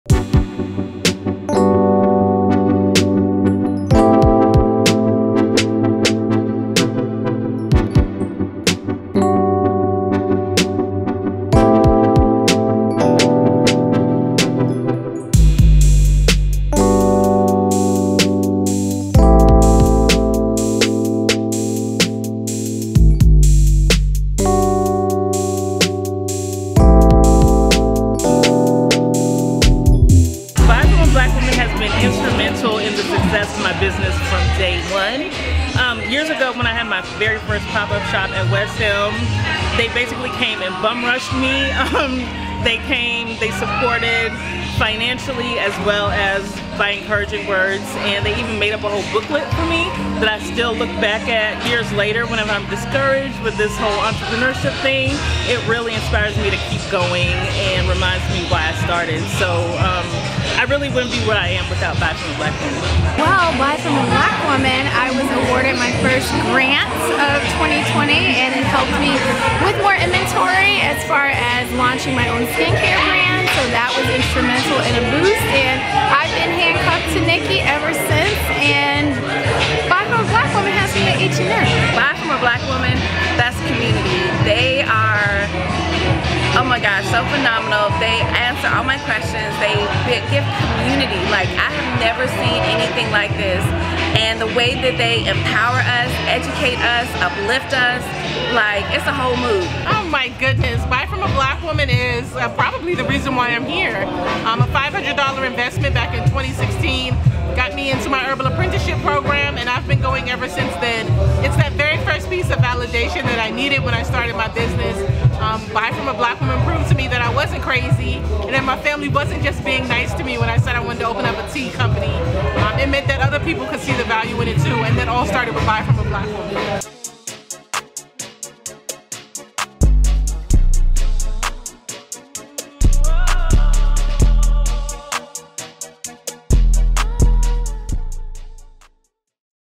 The Buy From A Black Woman Holiday Market | Week 2
On Dec. 4, The Buy From A Black Holiday Market presented by H&M was back for Week 2! Listen in as we ask the business owners featured this week, how has Buy From A Black Woman helped them with their business.